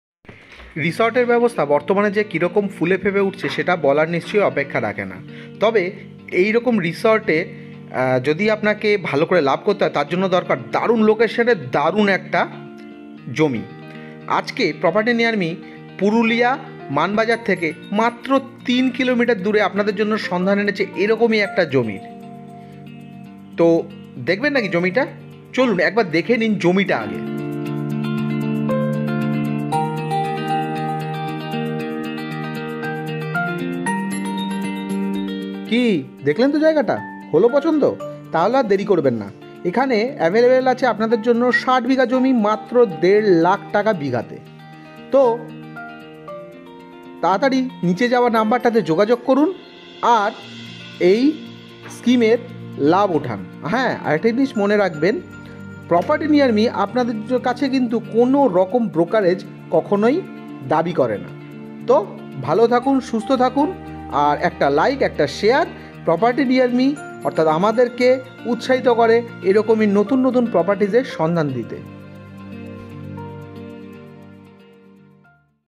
Sight for Resort... 1.5 Lakh only... রিসর্টের জমি এরকম জায়গায় মাত্র ১.৫ লাখে...
রিসর্টের ব্যবসার জন্য এইরকম জায়গা আর পাবেন না।তাও মাত্র ১.৫ লাখে (Without any brokerage). ভিডিও দেখলেই পছন্দ হয়ে যাবে...